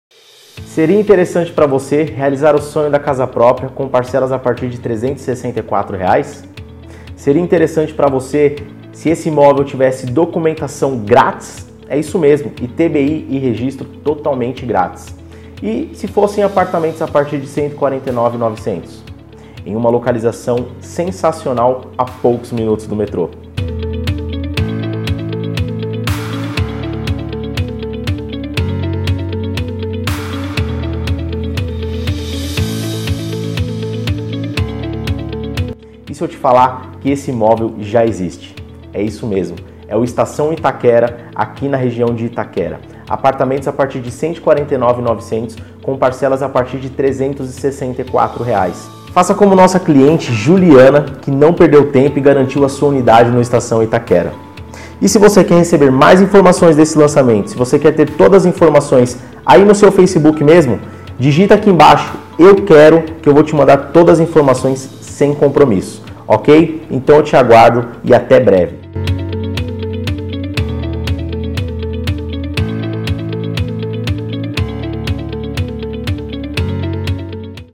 Apartamento em Itaquera a partir de R$149.900,00
Já pensou em 🏠 morar perto do 🚅Metrô, de comércios, escola e de áreas de 🌴 lazer como parques e shoppings pagando uma parcela a partir de R$364 e entrada que cabe no seu bolso❔ 🏡🏡🔑🔑
➡Documentação grátis (ITBI+Registro).;
➡Excelente área de lazer;

CRECI: J-25369